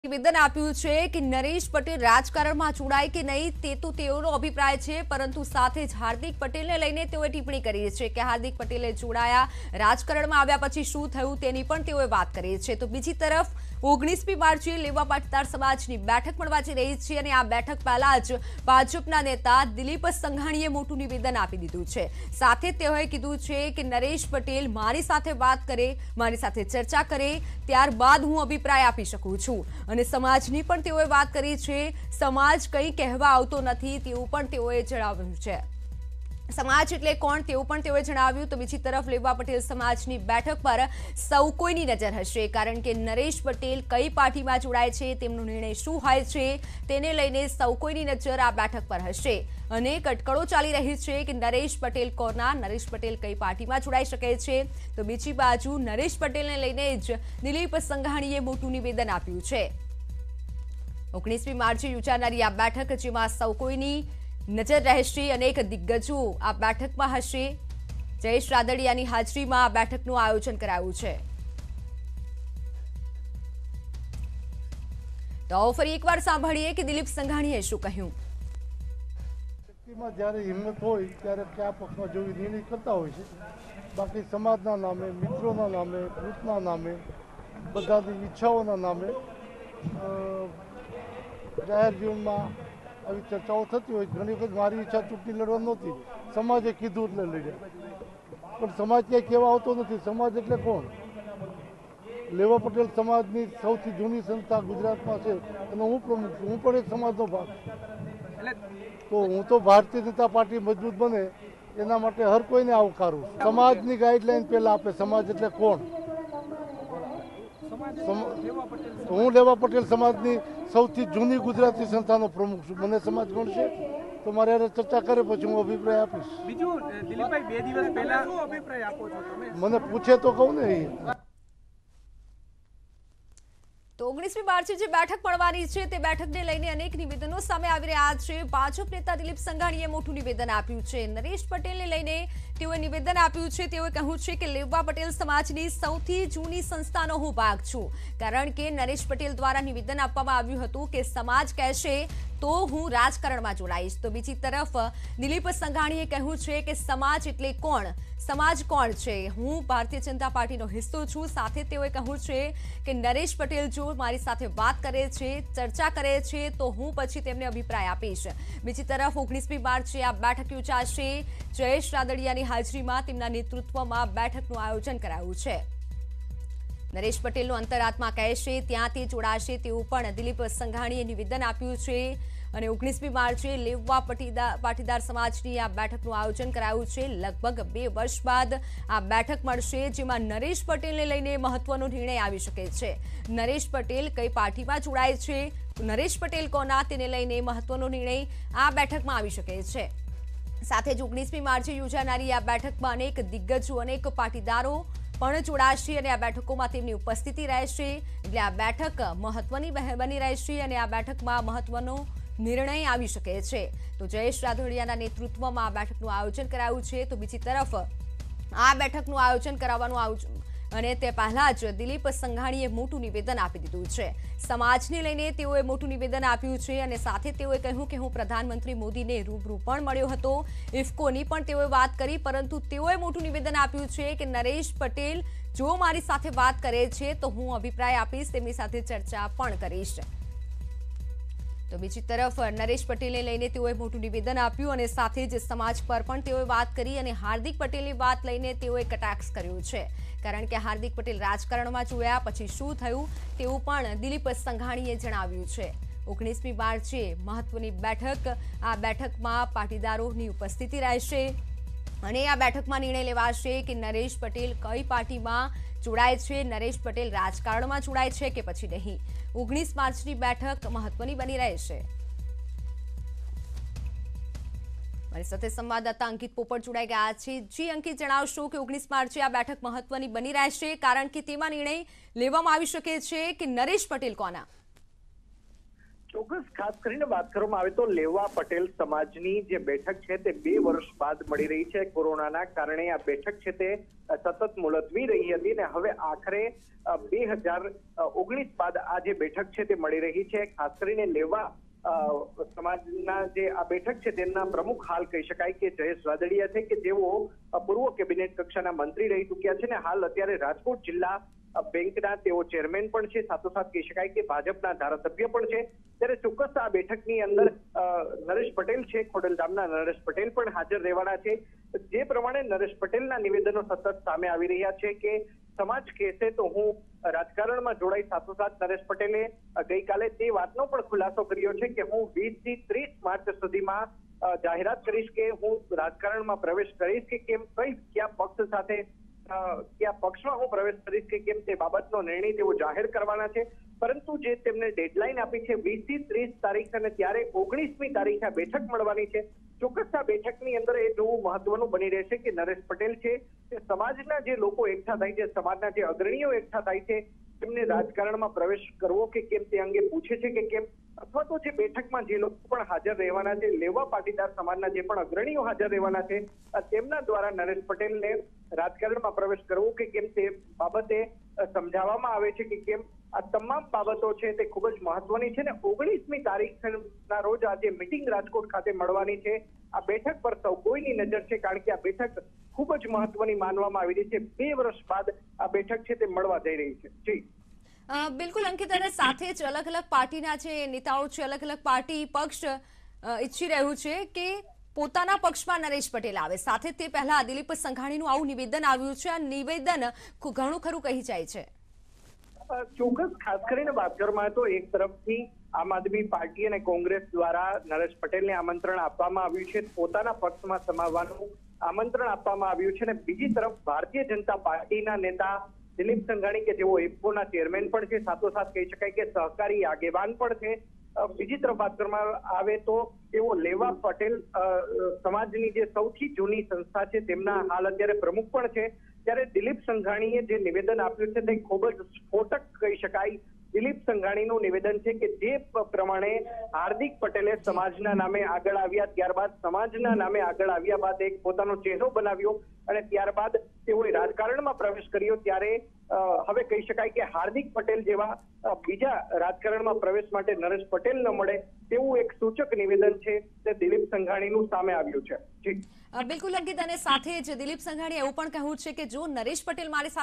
સમાજ કંઈ કહેવા આવતો નથી : Dilip Sanghani | Naresh Patel | News 18 Gujarati
ભાજપના નેતા Dilip Sanghani  એ મોટું નિવેદન આપ્યું છે અને કહ્યું કે Naresh Patel રાજકારણમાં જોડાય કે નહીં એ તો તેમનો પોતાનો અભિપ્રાય છે એમાં સમાજ કંઈ કહેવા આવતો નથી.